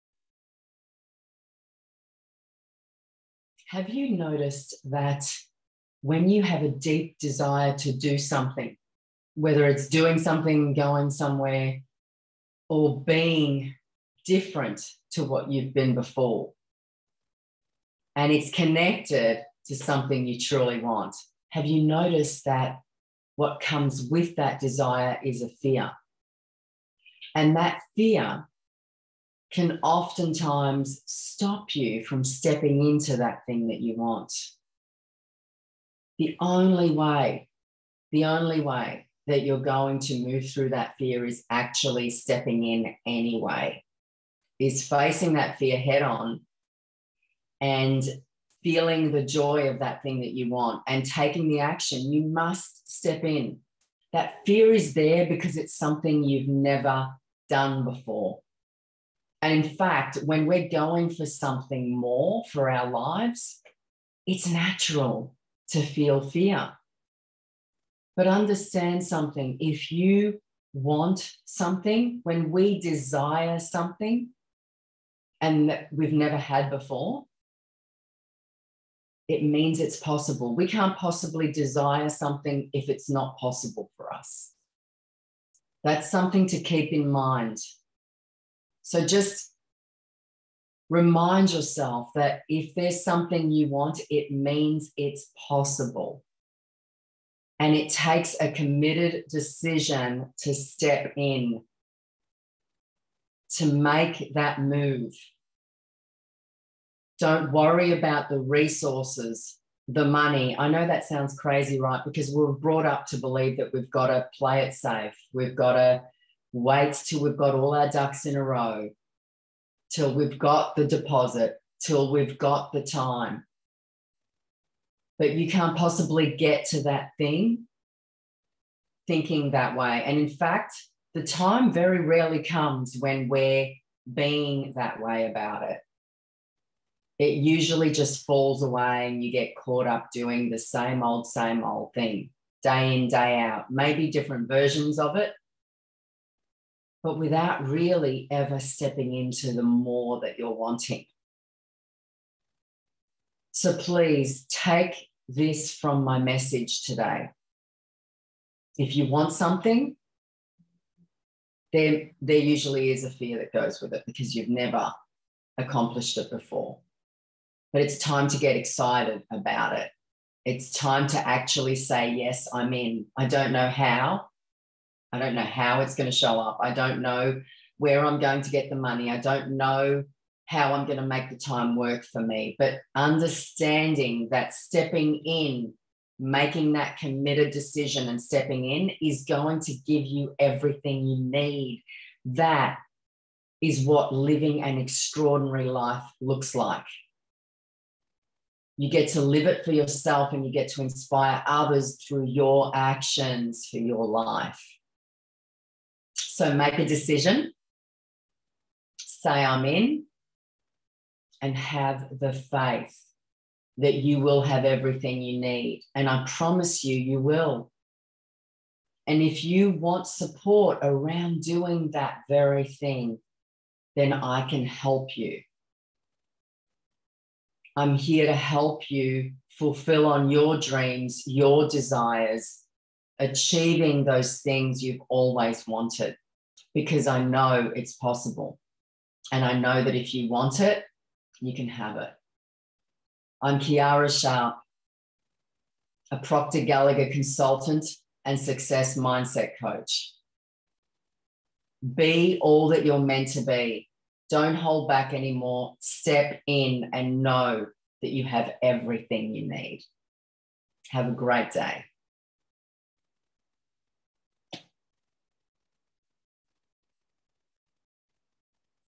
Feel The Fear and Step In to What You Want Anyway
Often times we don't move towards what we want because of some kind of fear...fear of looking silly, fear of failing, fear of not having enough time, money or resources.  The reasons are endless and they're there to hold us back.  It's called a PARADIGM.  Those negative beliefs and habits that keep us stuck and only going so far in life.  It's time to fly! It's time to get excited about what it is you want for your life and only focus on seeing that outcome, live it in your mind and take the action of DOING IT.  Know you will have everything you need to support you ONLY when you STEP IN. That's when the way SHOWS UP.
Decide.
Step in.
Have Faith.
You will have everything you need.